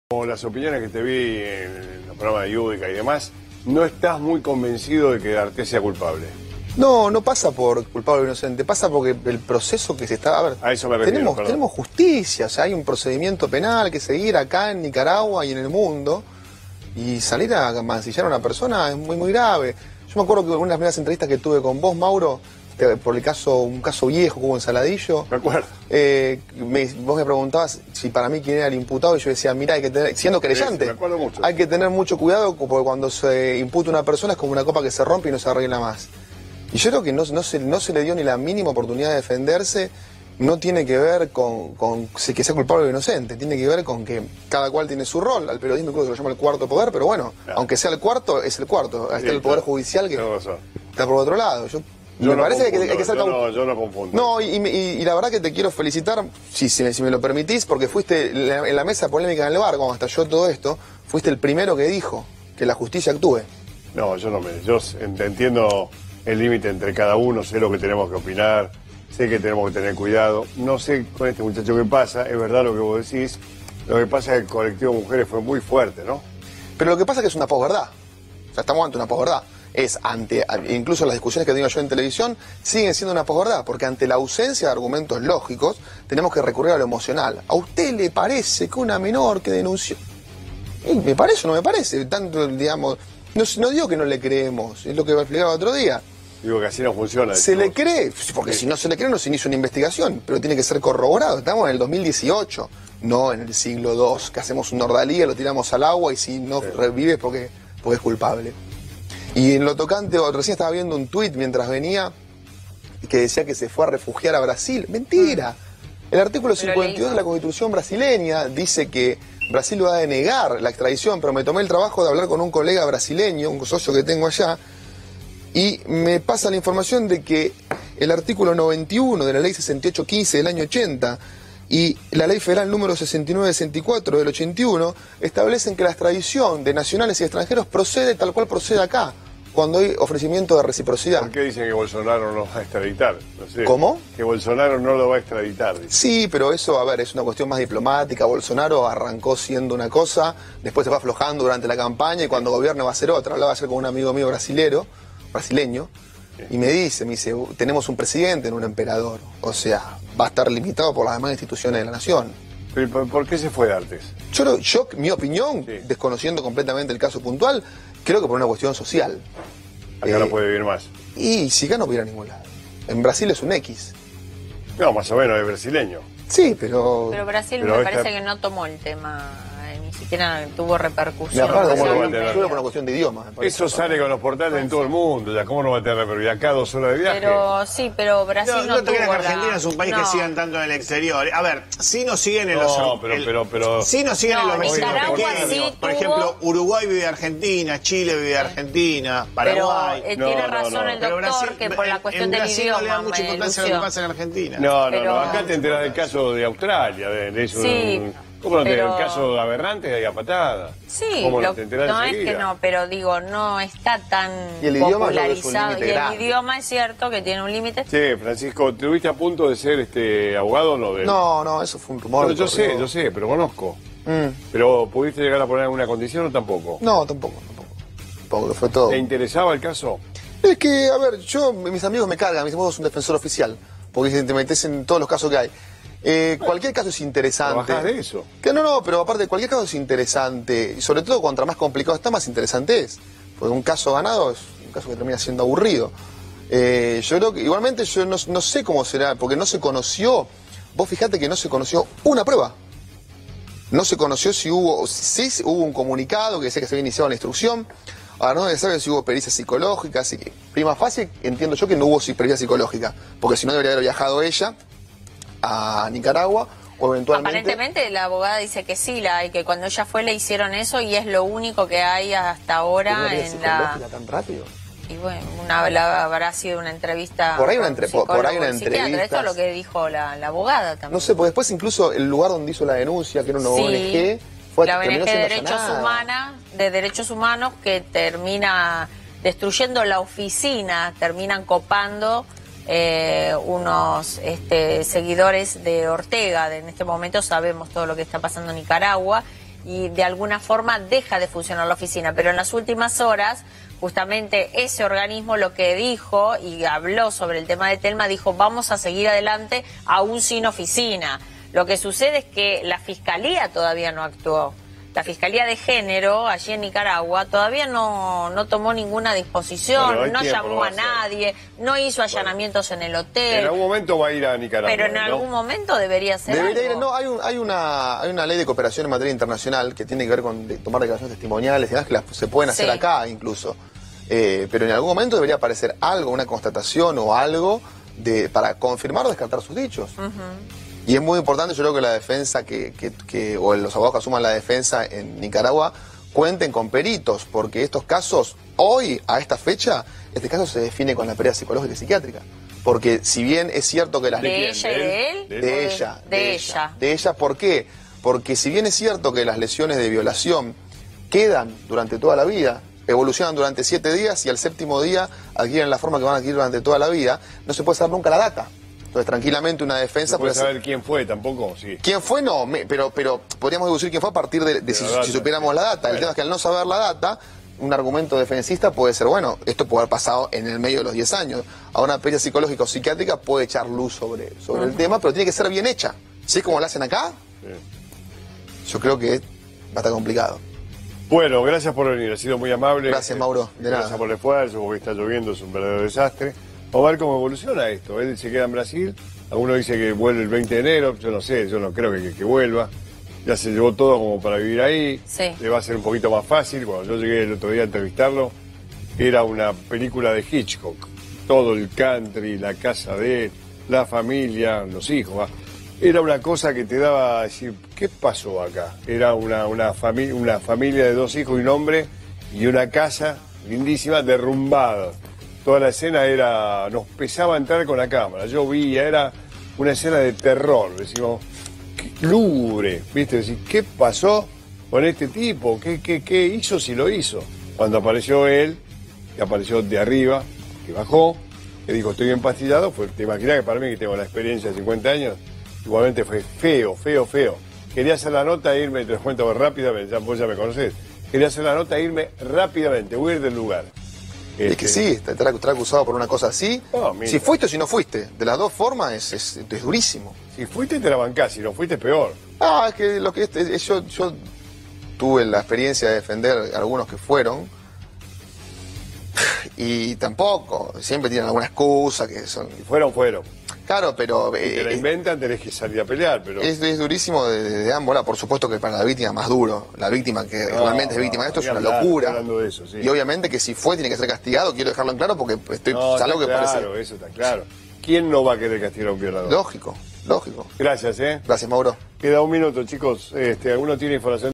Abogado nivel dios - Francisco Oneto Con Mauro Viale en A24 entrevista exclusiva
Francisco Oneto paso hoy por A24 a aclarar los puntos sobre el caso de Juan Darthes, ahora refugiado en San Pablo - Brasil escapando de la condena mediática con sus dos hijos.
Todavia no esta claro